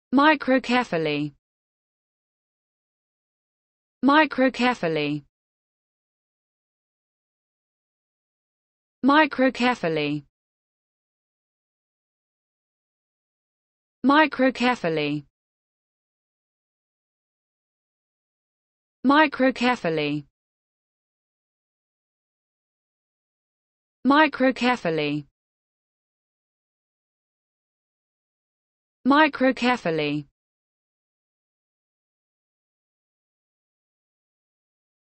the correct pronunciation of microfilming in English.
This video shows you how to pronunciation microfilming in English.